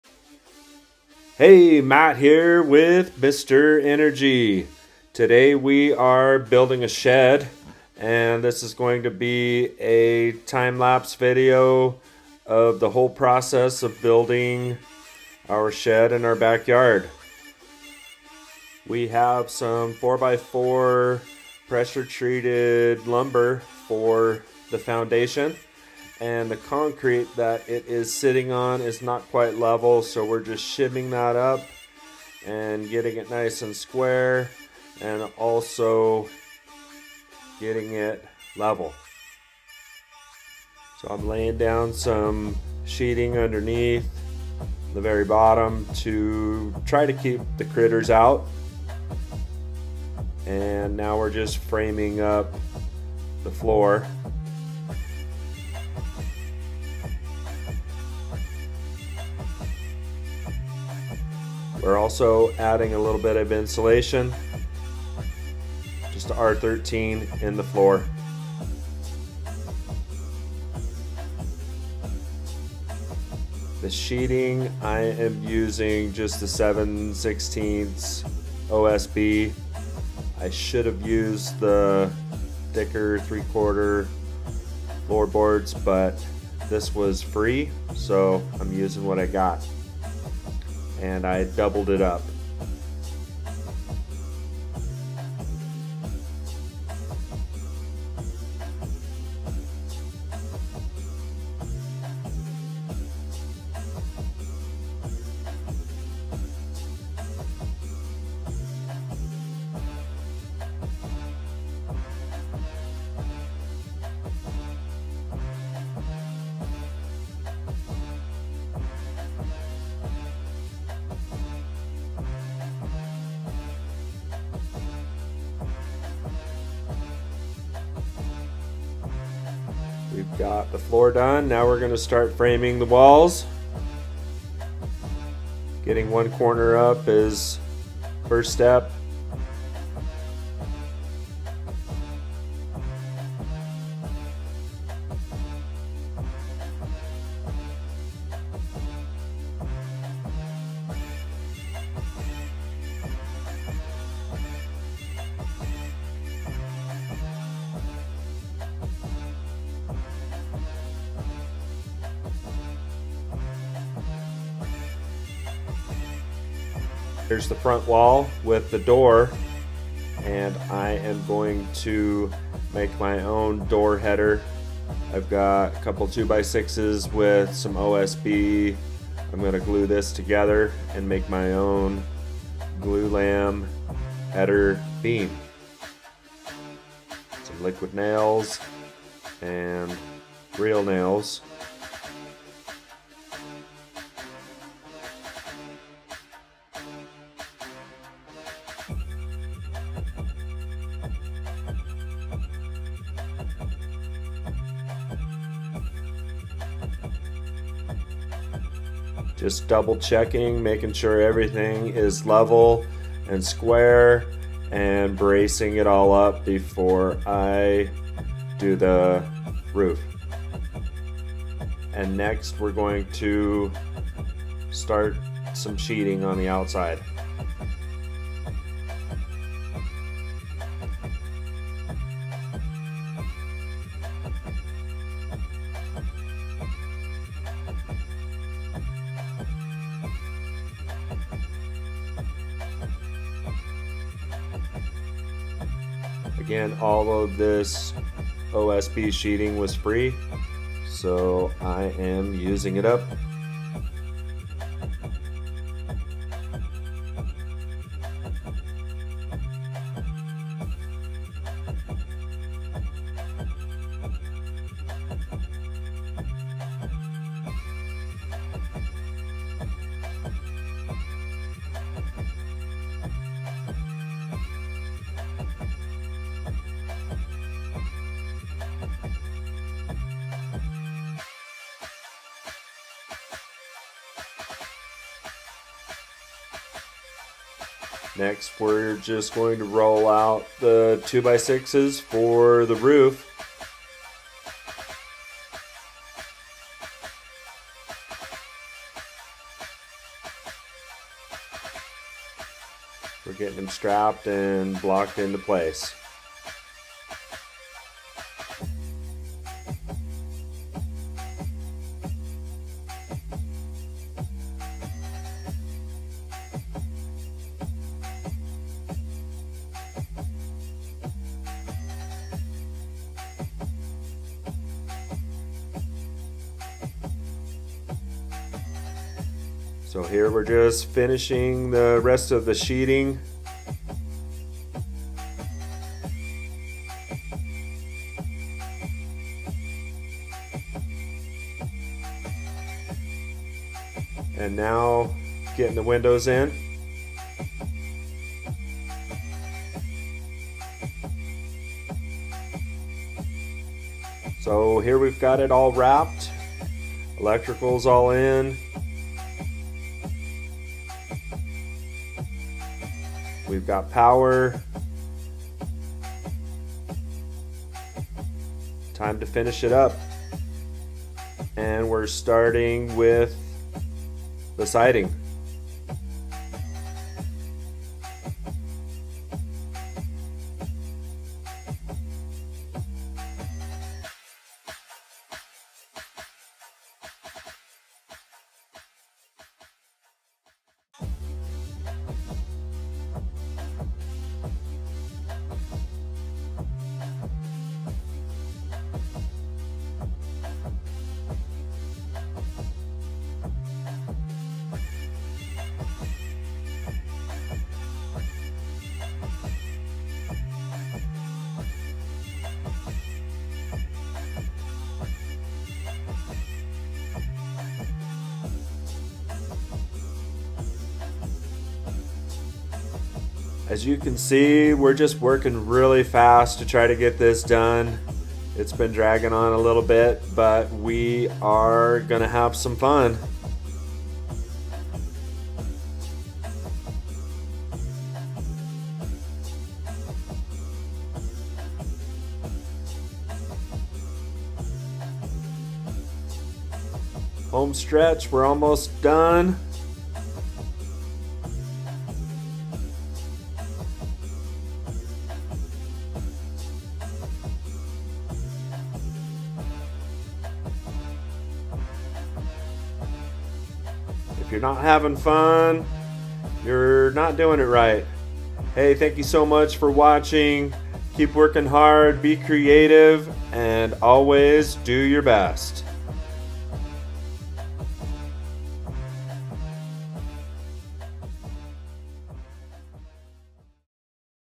From Dust to Dream: The Ultimate Shed Timelapse Spectacle!
🏠 Welcome to the ultimate shed-building experience! 🛠️ Join us on this mesmerizing journey as we transform a pile of materials into a shed through the magic of time-lapse! 🌟

In this captivating time lapse video, witness the entire process unfold from the ground up. From laying the foundation to hammering in the last nail, you won't want to miss a single frame of this DIY masterpiece.

🎥 Watch as our skilled team brings creativity, craftsmanship, and dedication to life, turning a vision into reality. Whether you're a DIY enthusiast, a shed-building novice, or simply looking for some satisfying content, this time lapse has something for everyone.

🛠️ Explore the intricate details of construction and gather inspiration for your own projects. It's more than just a shed—it's a testament to hard work, passion, and the joy of creating something with your own hands.

🔧 Ready to embark on your own building adventure? Don't forget to leave a comment to share your thoughts and ideas. Let the shed-building journey begin! 🌈✨